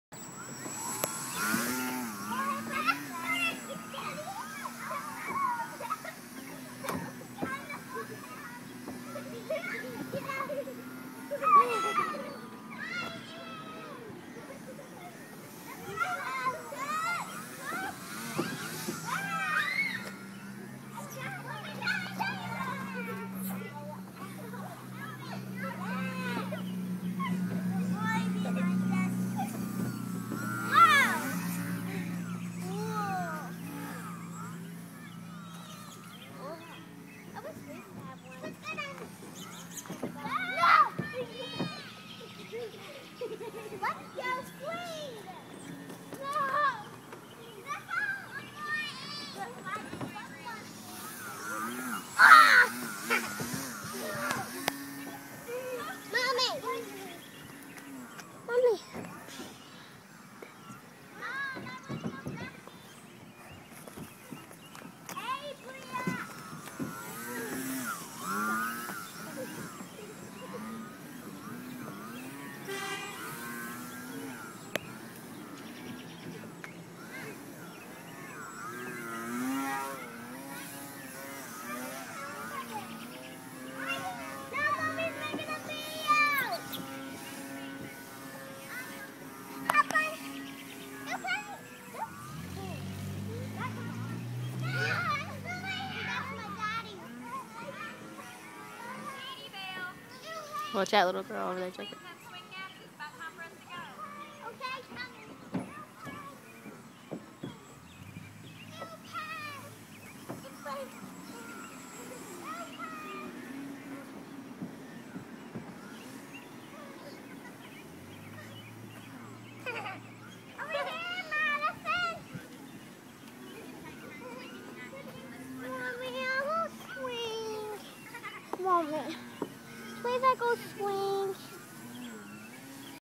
LightRC Magnum 33" Lite
Maiden flight at the park. Setup recommended by Sam Steeb.